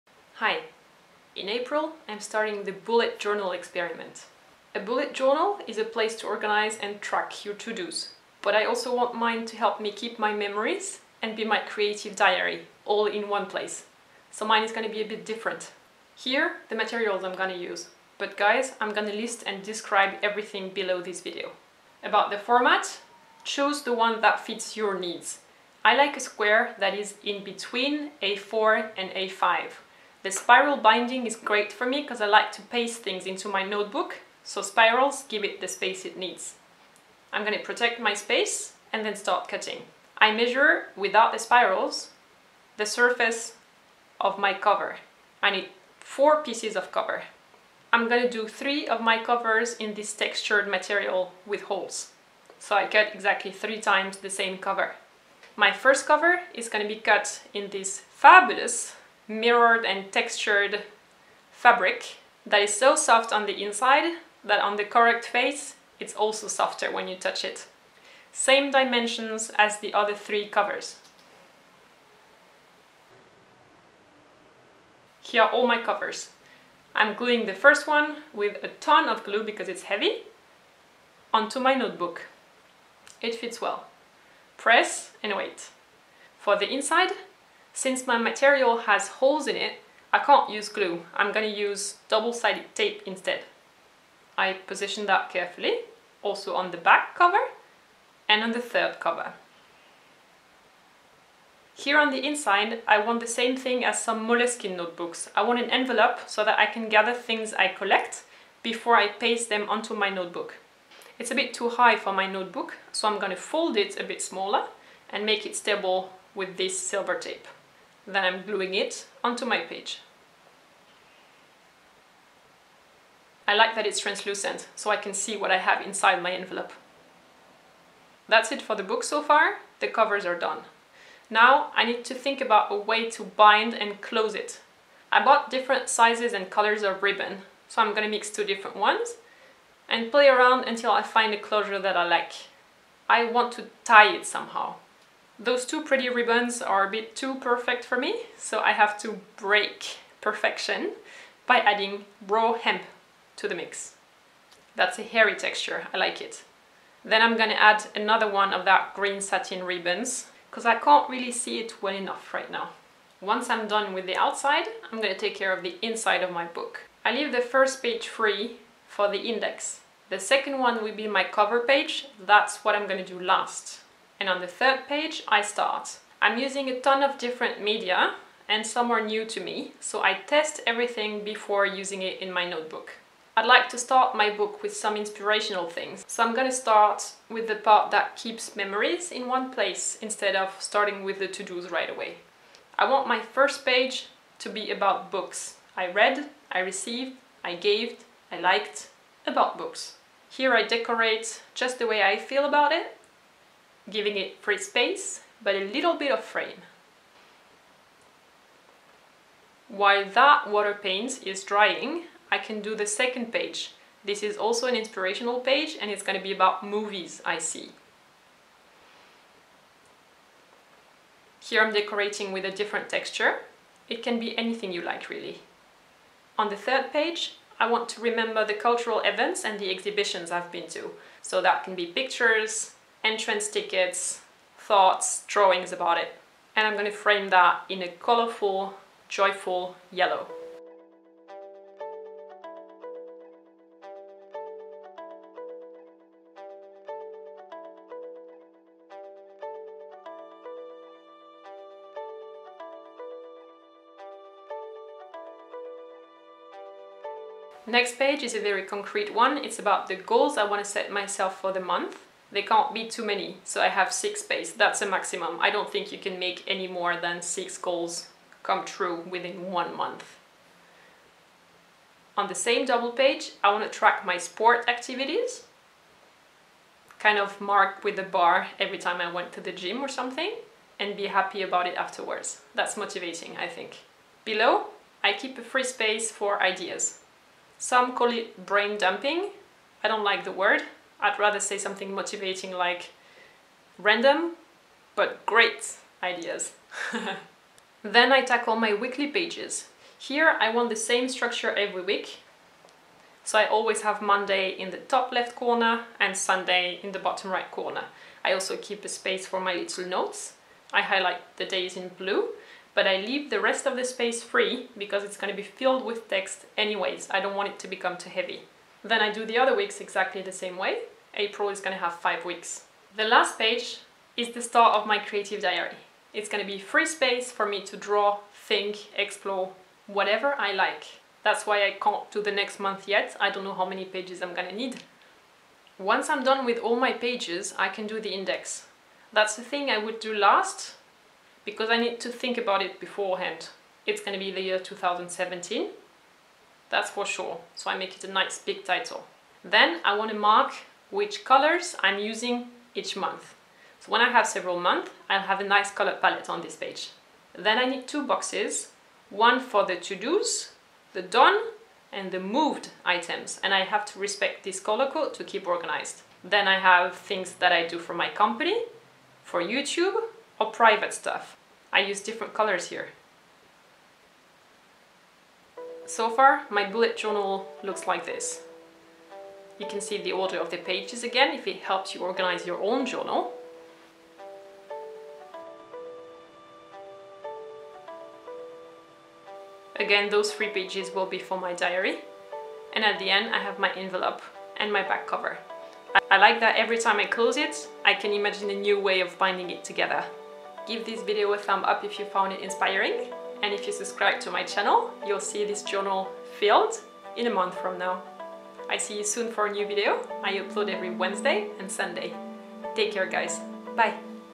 Creative bullet journal: how to start & initial setup | Justine Leconte
A bullet journal is a very personal thing. I decided to start bullet journaling and gather my to do's, my memories and my creative diary all in one place. This tutorial shows how to make your own journal and how to do the setup and organize it. If this works out for me, I'll show you my bullet journal in a run-through at the end of the month!
Plan with me: you have time until the end of March to setup your own bullet journal, so you can start April together with me if you wish!

New videos: Wednesday 17:00 & Sunday 12:00 (Paris/Berlin time).

How you watched my video from last week about French style in 10 tips?

Take care!
Justine

MATERIALS I USED:
- Paper, fabric or plastic to build your covers
- White pages and a binding (or a ready-to-use notebook with spiral binding)
- Something to make the closure (e.g. ribbons to tie, sew, cut, button, zipper...)
- Tape, double-sided tape, crafts glue, fabric glue, all-purpose glue, scissors, metal ruler, cutter, cutting mat
- Pens: I used black ink by Faber Castell and Micron (0.05mm and 1mm)
- Watercolor and brushes with a water reserve so that you don't have to dip a brush into the water anymore
- Water-based markers (don't use regular design markers because they will bleed through your pages if they are not maker paper pages!)
- Decoration tape in several colors
- An envelop to attach to your notebook if you wish
... and any other materials you'd like to use or try!
Your bullet journal is your chance to experiment with new media :-)

SOCIAL